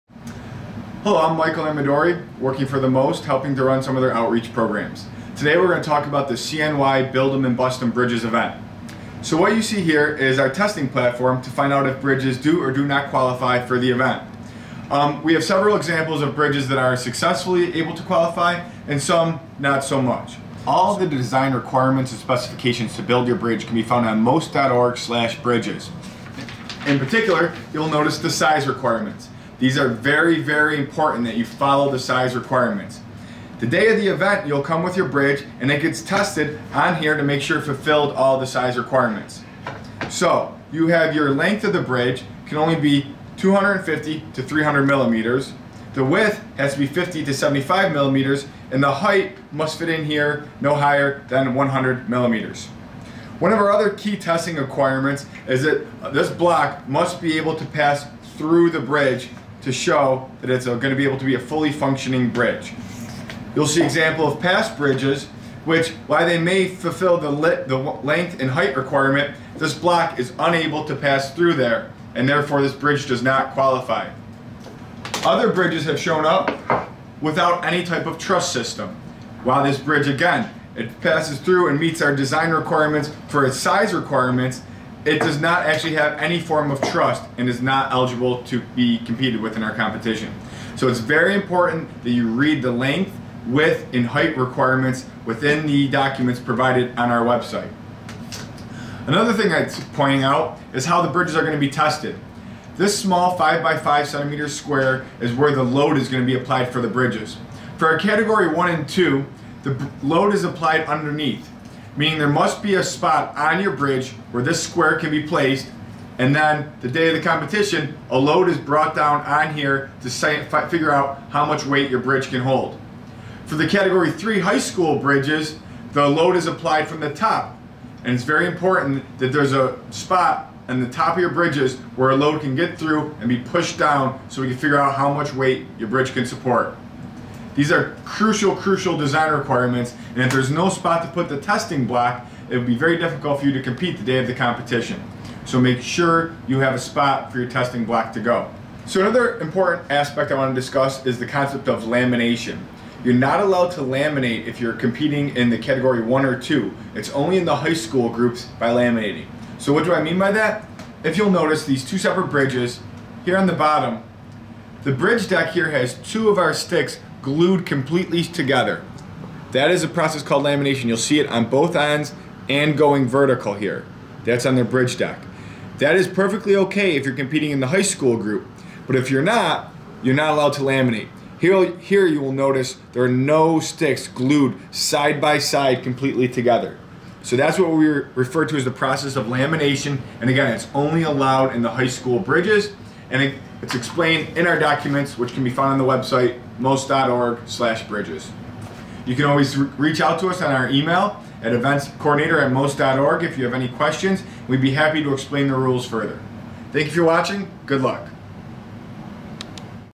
Inspection Criteria for Bridges Build 'Em & Bust 'Em at the MOST
This video demonstrates the inspection that entries must undergo to participate in the Museum of Science & Technology's CNY Bridges Build 'Em & Bust 'Em Competition. It's a great visual tool, even if you have participated in the past.